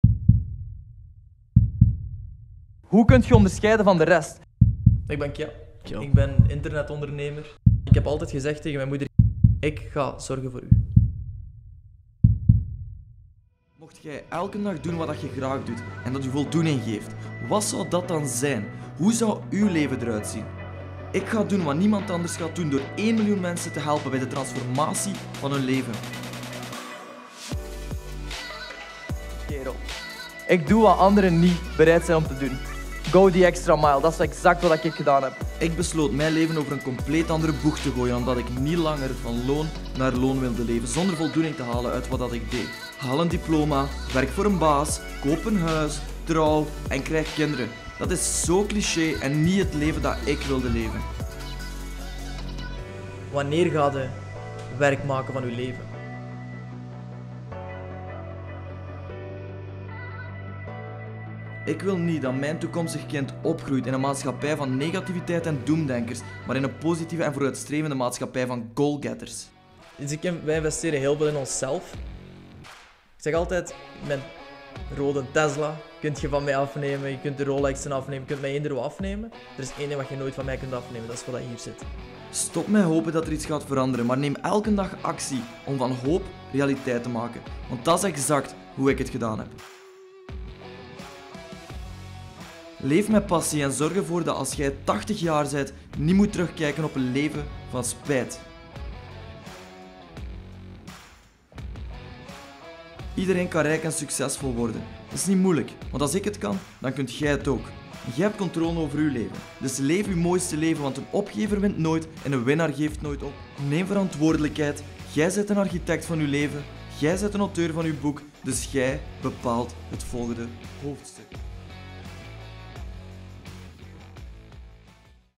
Wie is Kjell Fruytier?
Ik ben dit kanaal gestart om mijn reis te documenteren zodat jij gemotiveerd en geïnspireerd raakt.

Mijn missie: Één miljoen mensen hun leven veranderen door hun te motiveren en inspireren naar de beste versie van zichzelf.

Hoe kan je mij volgen?